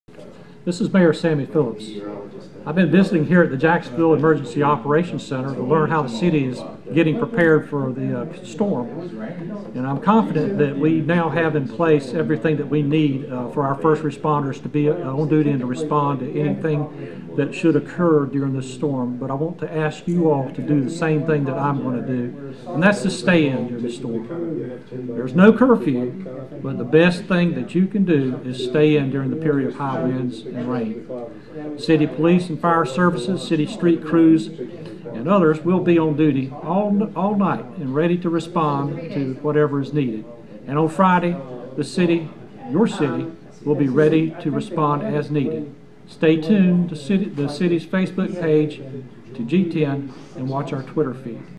Special Message From Mayor Sammy Phillps
Special message from Mayor Sammy Phillps regarding Hurricane Arthur.